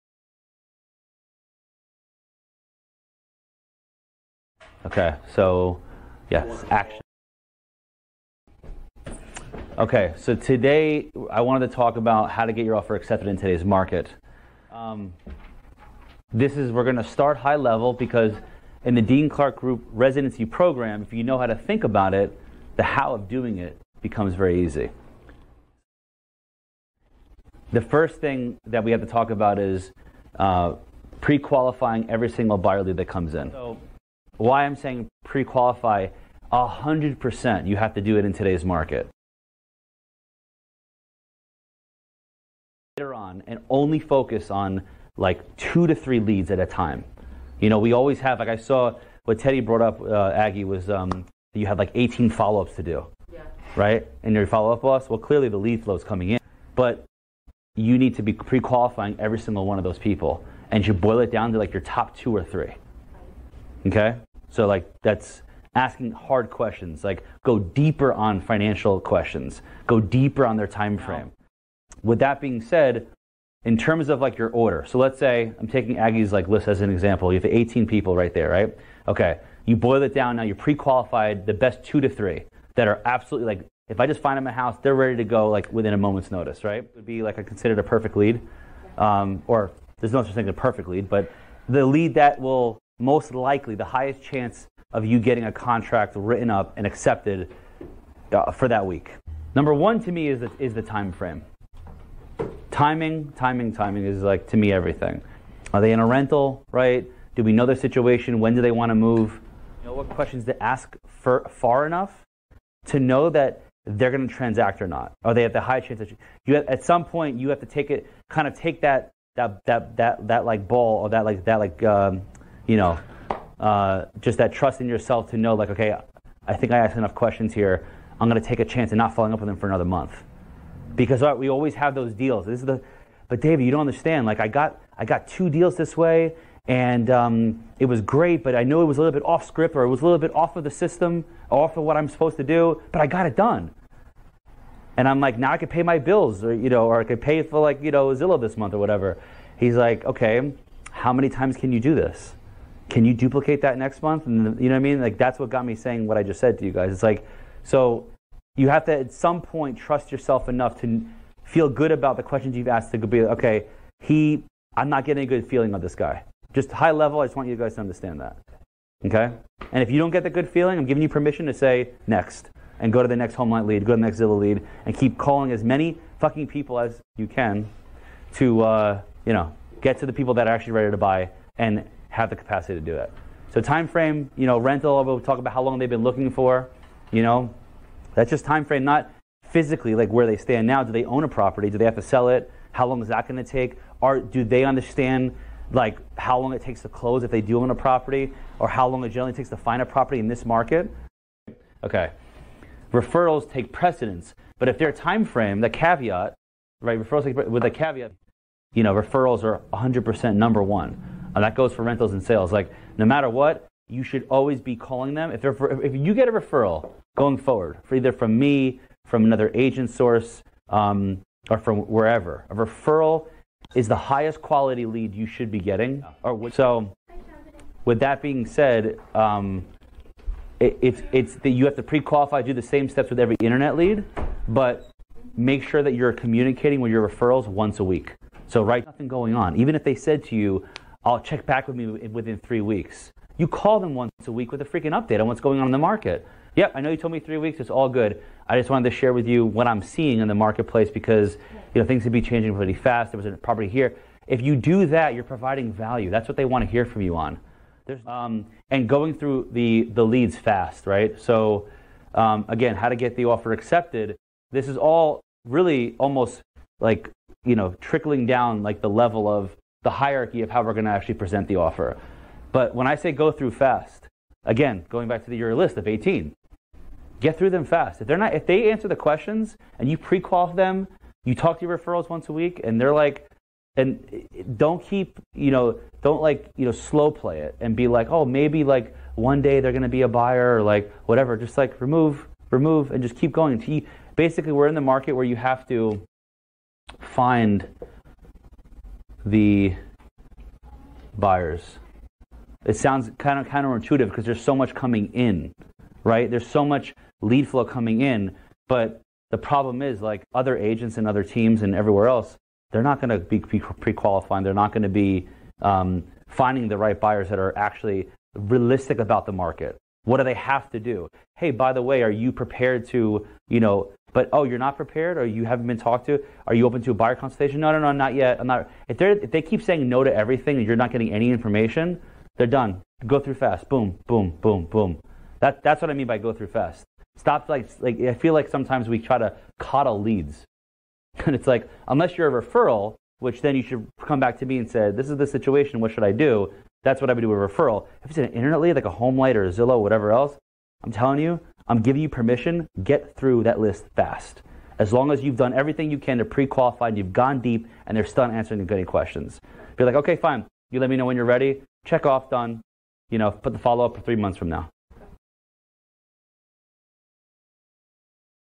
Residency Training Day Two: How Leads will Help You Close in Today's Real Estate Market
Leads are a big part of how real estate agents keep the market alive. Focusing on the right leads will get you one step closer to closing on a property with a prospective client.